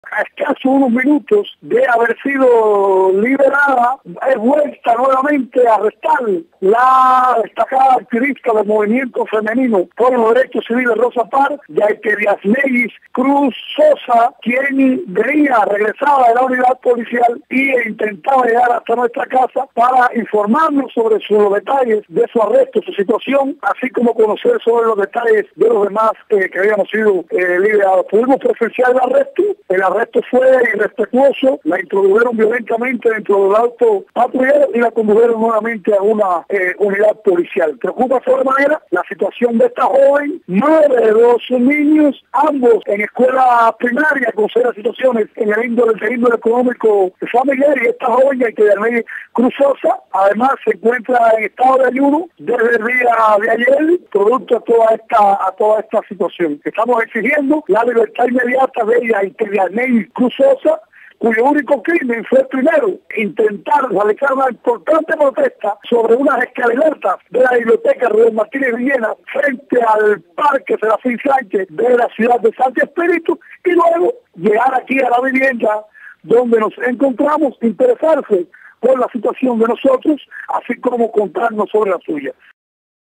Minutos después de ser liberada, activista es arrestada otra vez
(14 de enero de 2013) -- Apenas unos minutos tras ser liberada Yaite Cruz Sosa, del Movimiento por los Derechos Civiles Rosa Parks, cuando la policía política violentamente interrumpió una manifestación cívica en Sancti Spiritus, los mismos represores volvieron arrestarla, denuncio Jorge Luis García Pérez 'Antúnez'.  Más detalles en el audio.